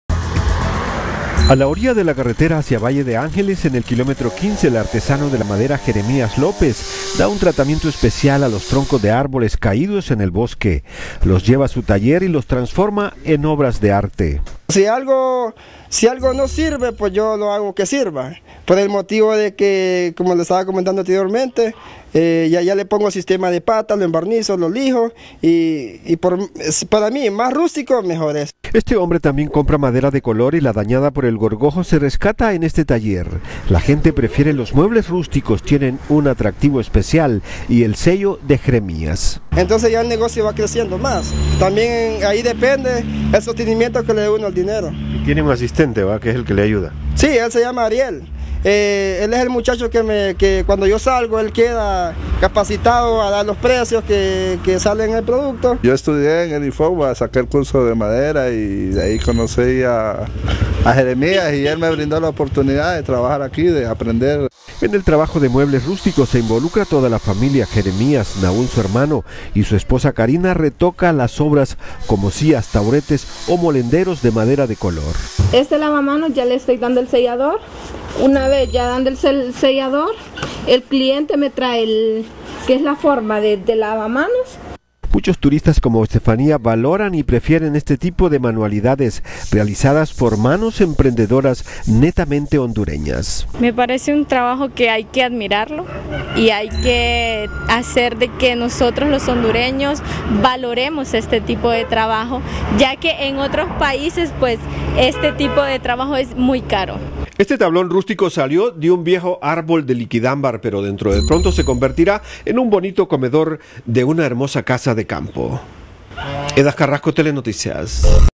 TN5 Estelar- Hermosas piezas talladas en madera camino a Valle de Ángeles
Este hondureño utiliza aquella madera que cae en el bosque y que ha sido afectada por el gorgojo descortezador.